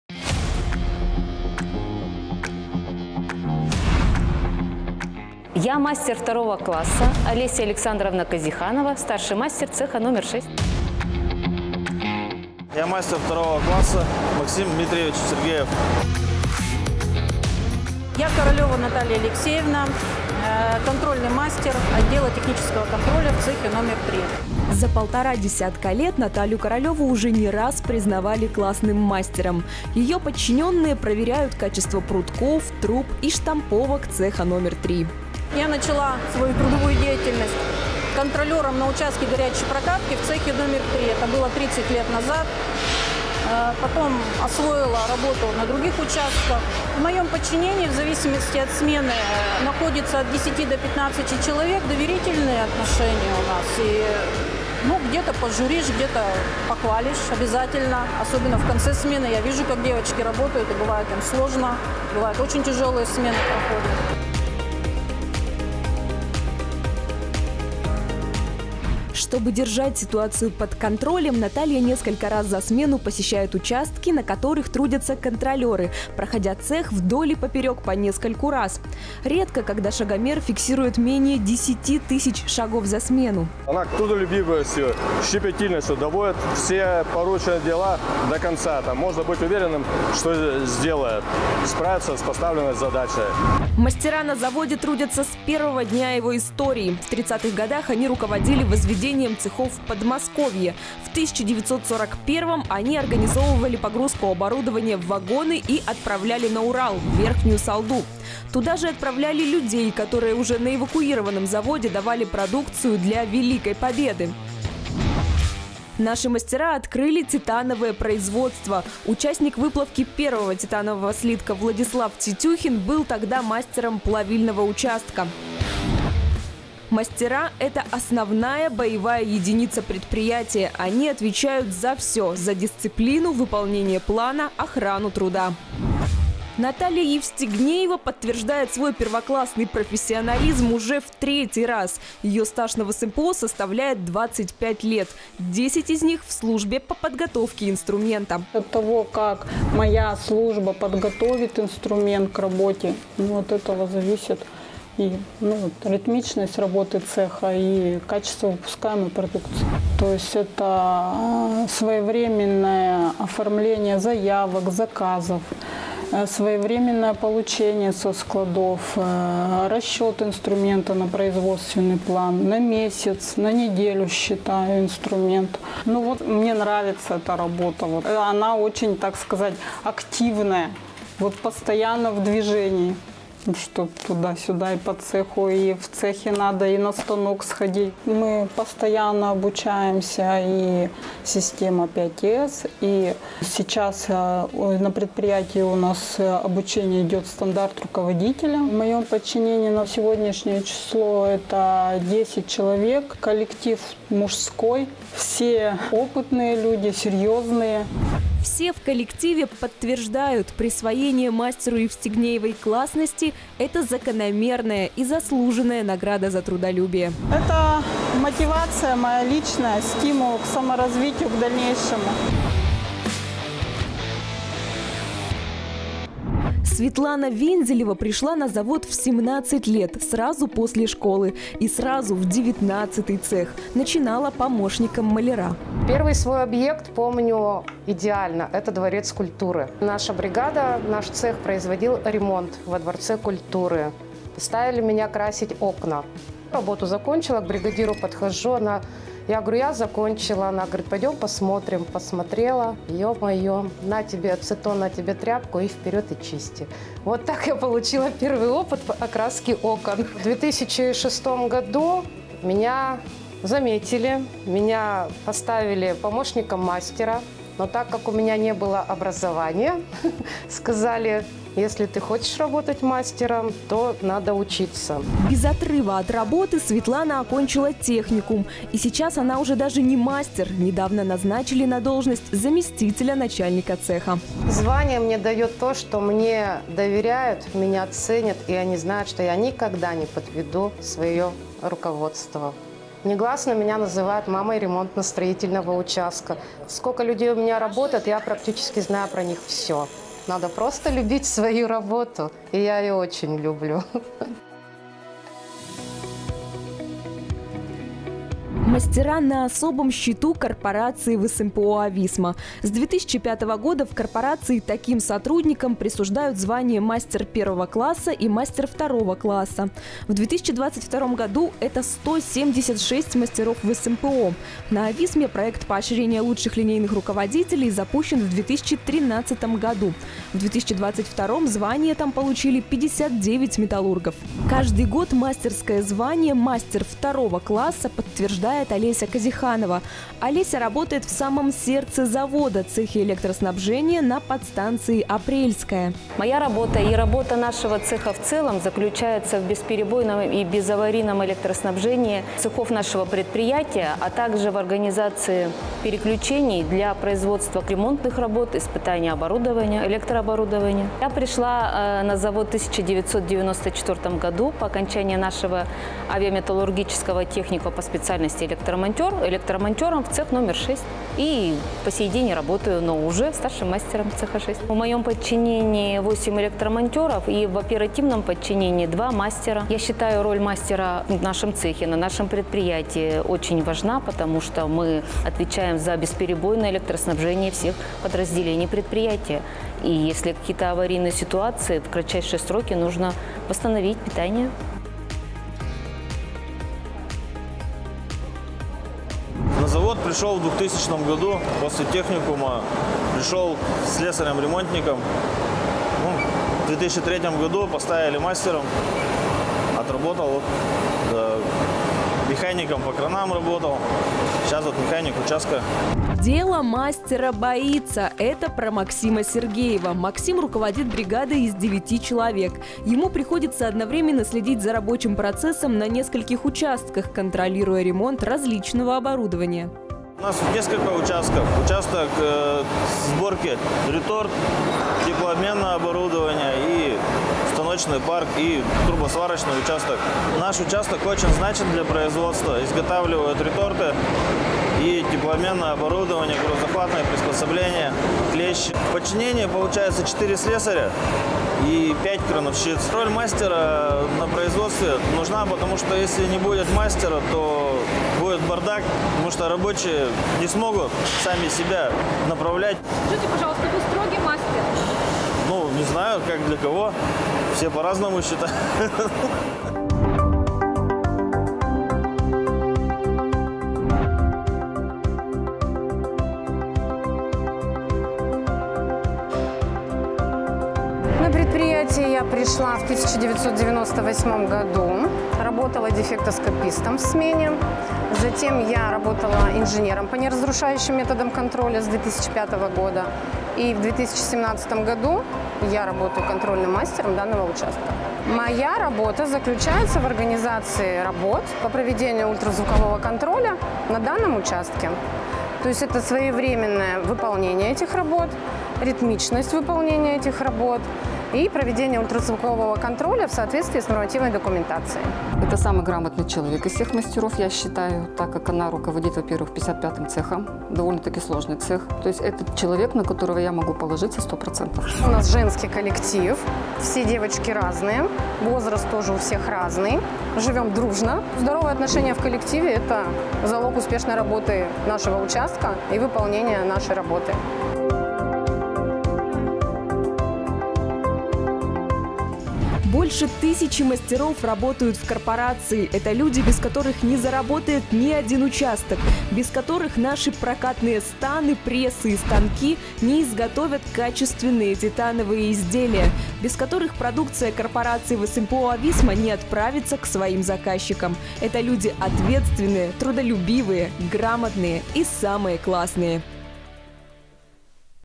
ЛУЧШИЕ МАСТЕРА КОРПОРАЦИИ
Сотрудникам Корпорации ВСМПО-АВИСМА вручили удостоверения о присвоении звания «Мастер первого класса» и «Мастер второго класса». К этому событию журналисты ВСМПО-АВИСМА Медиа подготовили фильм.